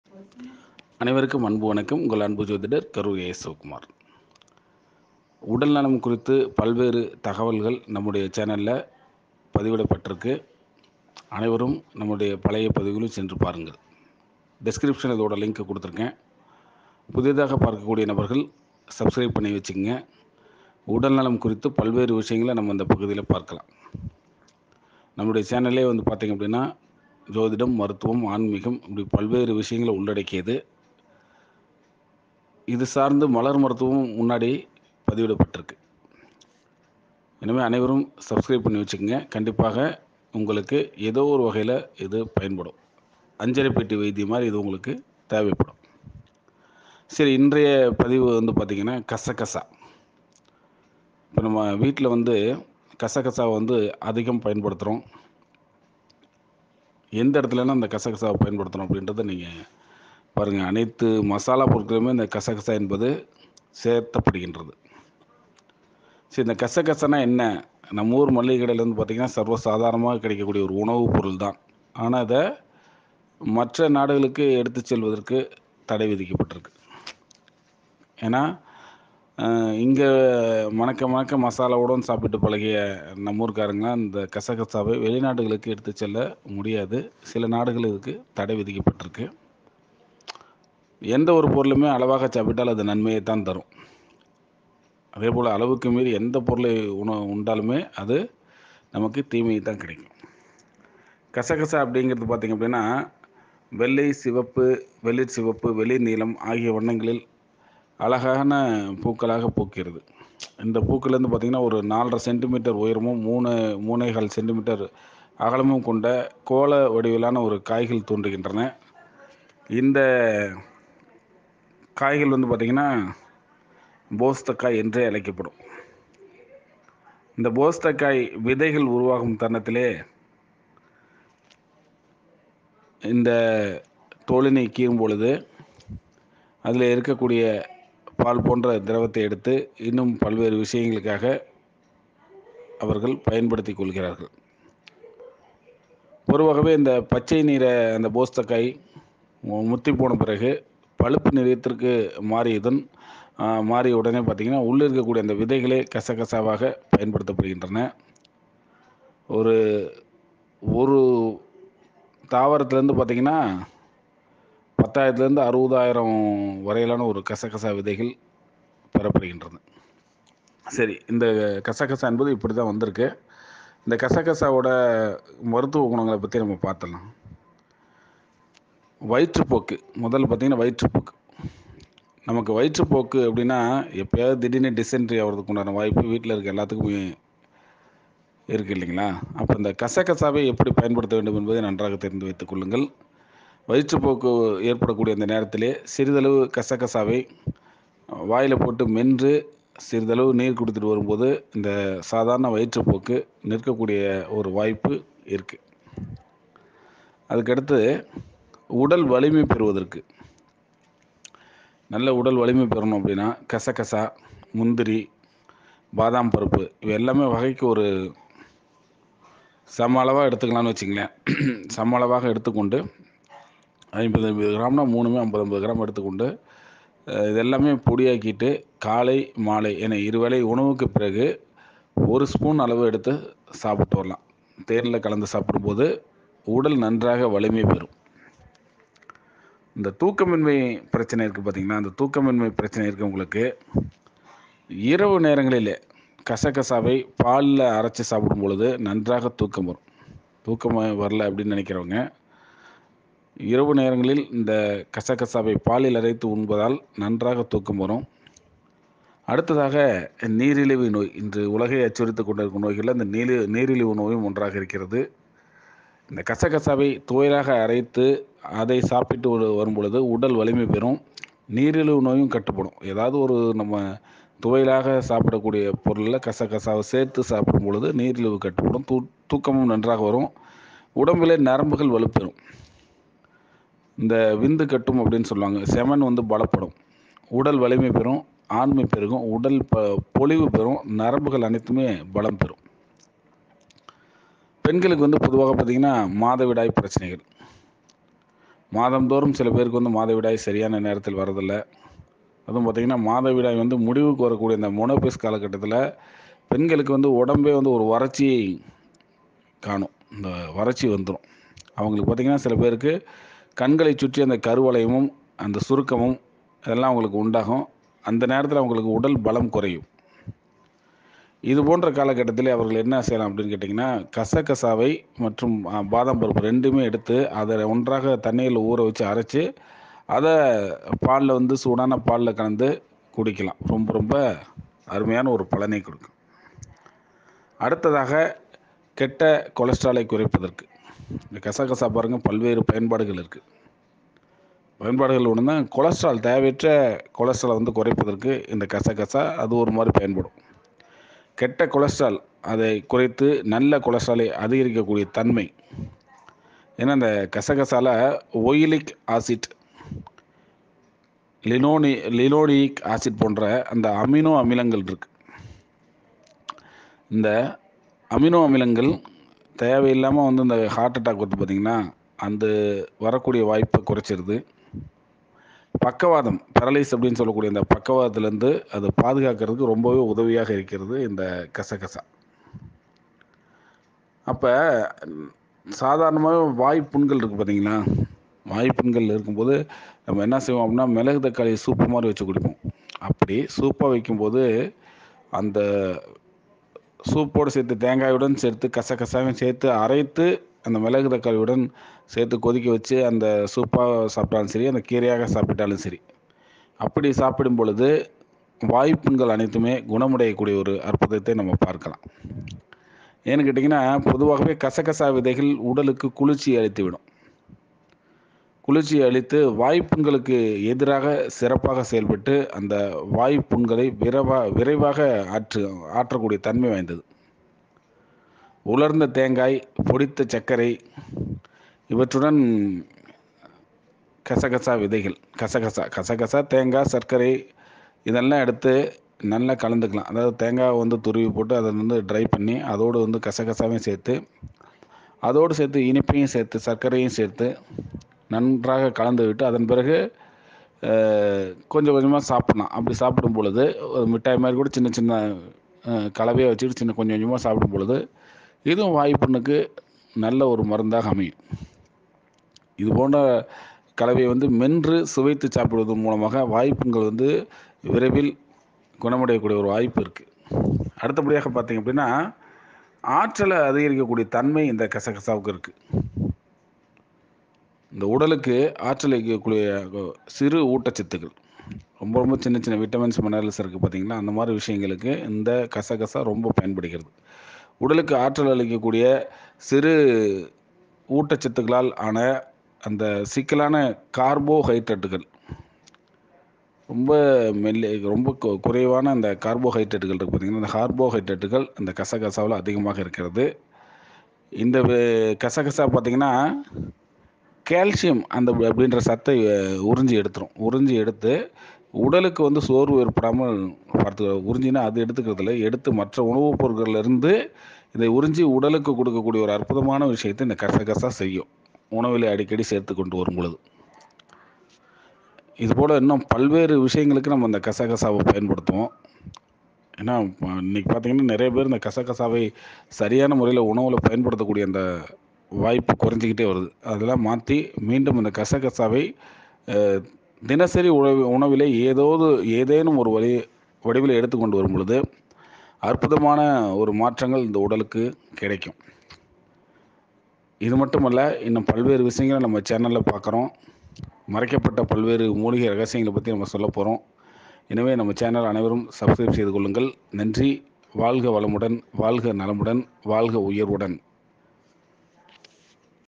கசகசா உடலுக்கு நன்மை செய்யும் கசகசாவை எப்படி உபயோகப்படுத்த வேண்டும் Poppy is good for the body
8  உடலுக்கு நன்மை செய்யும் கசகசா
உடலுக்கு நன்மை செய்யும் கசகசா இது எப்படி கிடைக்கின்றது இந்த கசகசா குணப்படுத்தும் நோய்கள் என்ன உடல் வலிமை பெற கசகசாவை எப்படி உபயோகப்படுத்த வேண்டும் அற்புதமான பதிவு கேட்டு பயன்பெறுங்கள்